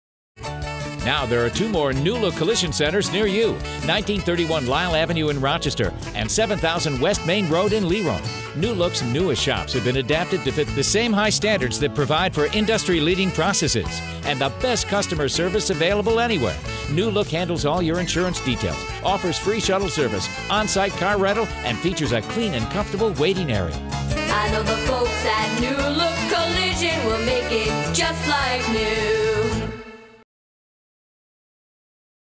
Nu-Look LeRoy and Nu-Look Lyell Avenue
Announcing the opening of two Nu-Look stores!
7000 West Main Rd in LeRoy, NY and 1631 Lyell Avenue in Rochester, NY. Same great service you've come to expect for the last three decades, just two more convenient stores to choose from!